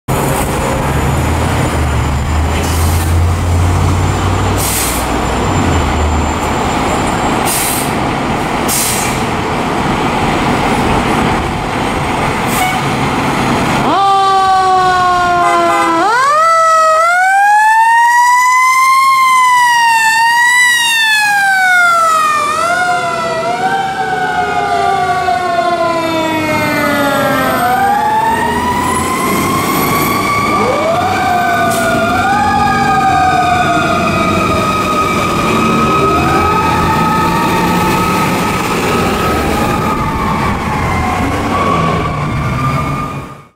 PFD Ladder 20 & Engine 36 Responding
Philadelphia Fire Department Ladder 20 & Engine 36 responding to a box assignment on Marsden Street.

Ladder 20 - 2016 KME 100'
Engine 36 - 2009 KME 1500/500 (EX-Engine 25)

Recorded: 2/2018